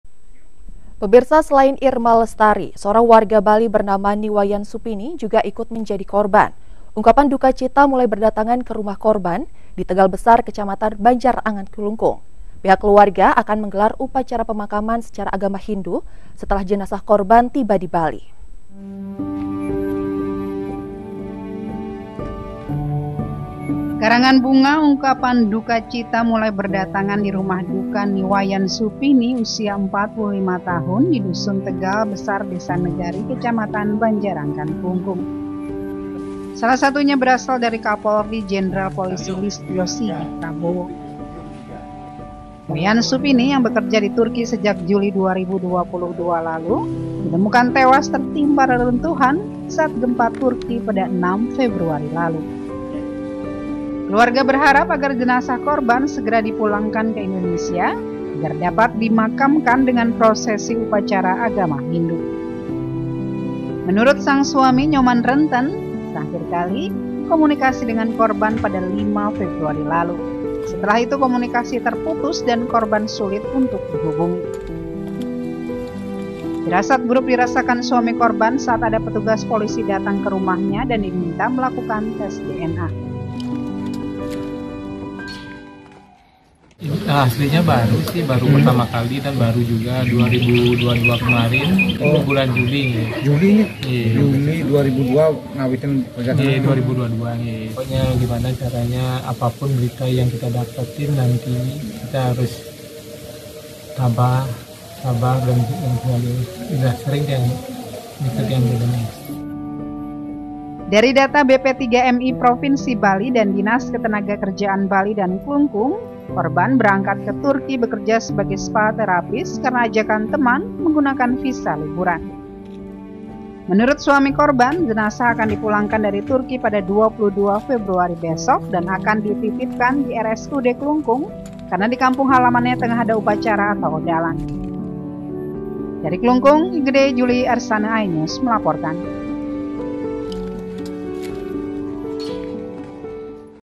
Keluarga Korban Gempa Turki Asal Bali Akan Gelar Upacara Pemakaman Secara Hindu
iNews Bali, 21 Februari 2023, Keluarga Korban Gempa Turki Asal Bali Akan Gelar Upacara Pemakaman Secara Hindu